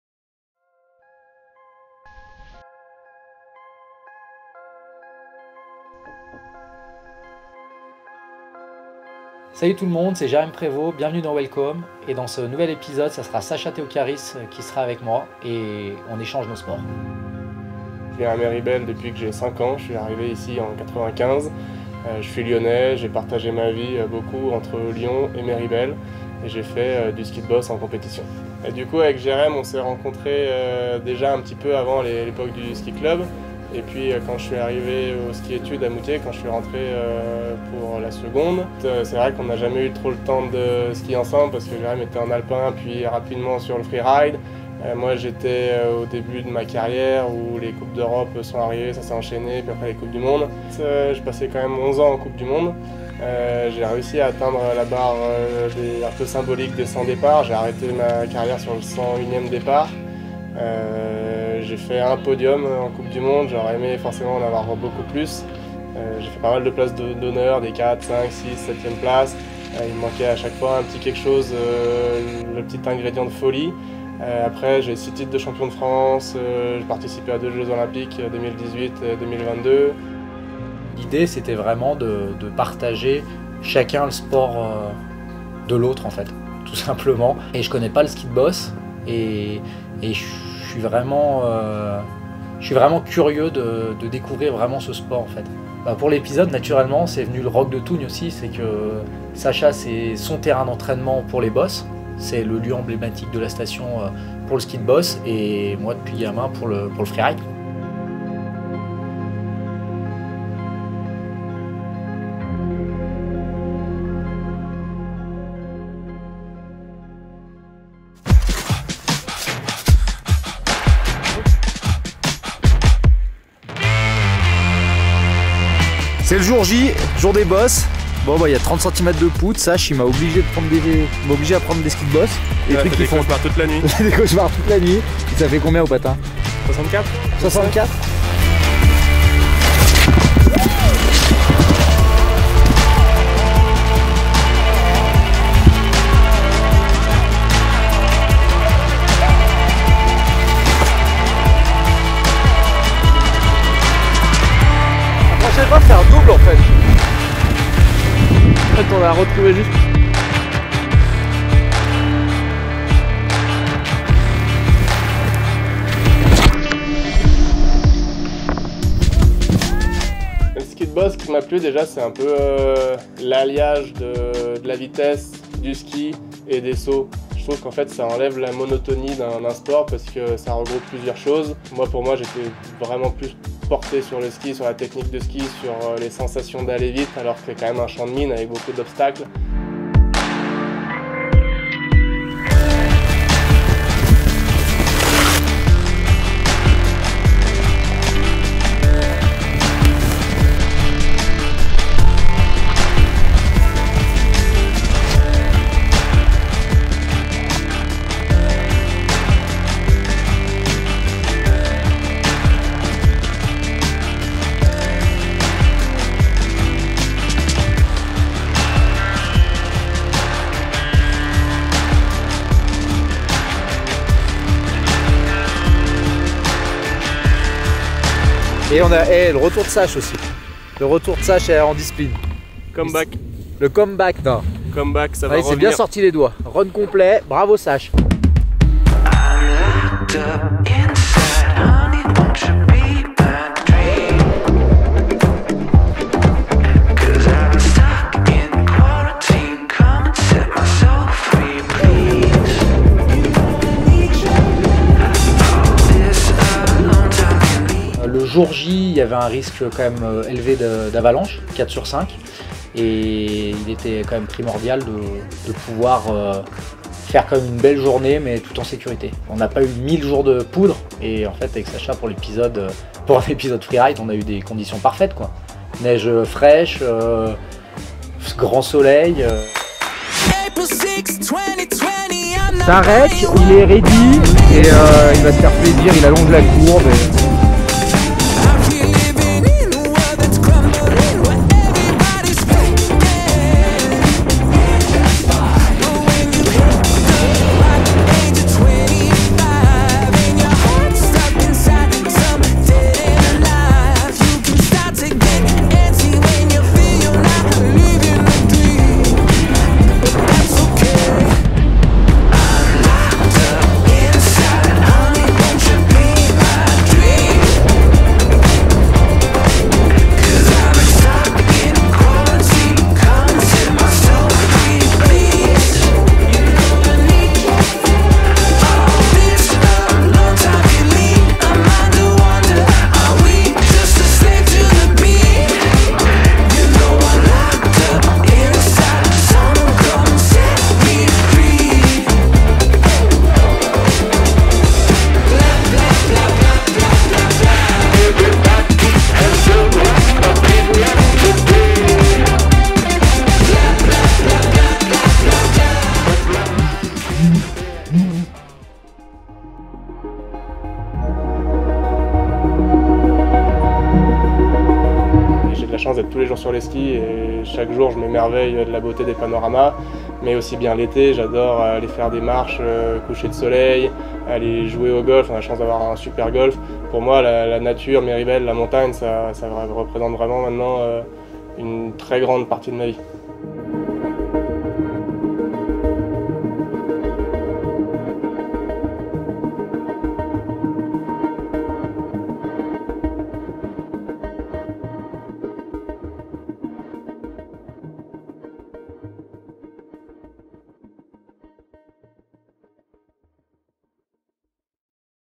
Welc[H]ome - Moguls X Freeride
Jeremy Prevost et Sacha Théocharis se testent dans leurs disciplines respectives : le freeride et le ski de bosse ! Un vrai challenge pour ces deux natifs de Méribel passionnés de ski mais champions dans des épreuves très différentes. Alors, vous êtes plutôt freeride ou ski de bosses ? Plutôt Sacha ou Jeremy ?
Jeremy Prevost and Sacha Théocharis test themselves in their respective disciplines: freeride and mogul skiing! A real challenge for these two Méribel natives who are passionate about skiing but champions in very different events. So, are you more a freerider or a mogul skier? More Sacha or Jeremy?